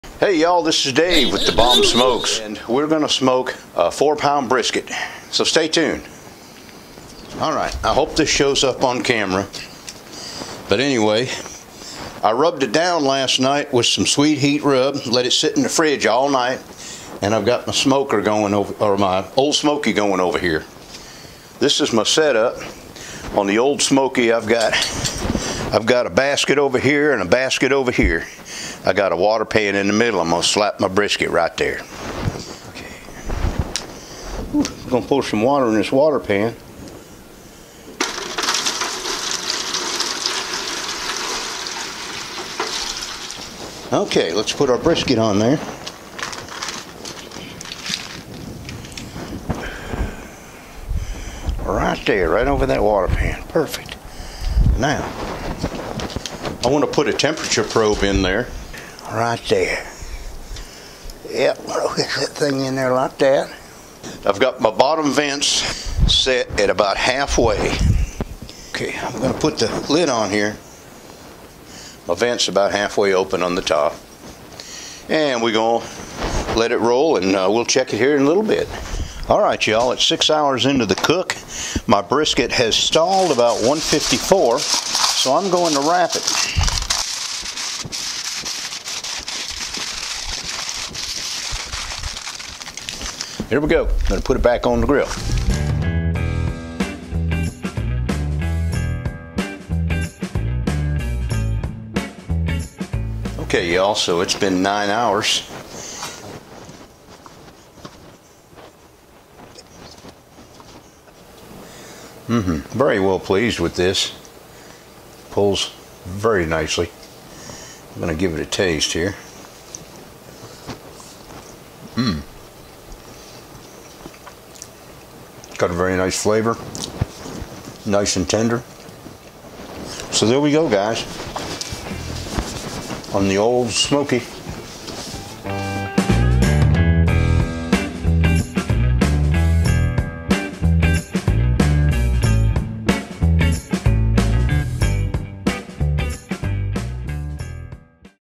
How To Smoke Brisket/Ole Smokey Grill
Have a 4lb Brisket and decided to smoke it using indirect method on my Ole Smokey Grill.
I rubbed brisket down the night before with some homemade Sweet Heat rub. Smoked it using Kingston Blue charcoal & Oak wood at 250-300deg until internal temp of meat reached 165F (6 hours). Pulled, wrapped, then smoked until internal temp reached 200 deg (another 2 hrs). Pulled and let rest for 1 hr before slicing.

Sweet Heat Meat Rub
1 cup Brown Sugar
1 Tbsp Paprika
1 Tbsp Garlic powder
1 Tbsp onion powder
1 Tbsp Chili powder
1 tsp Cumin
1/4 tsp Cayenne powder
1 tsp Black Pepper
1 tsp salt

Makes enough rub for about 3-4 pounds of meat.
Good on pork, beef, chicken, or turkey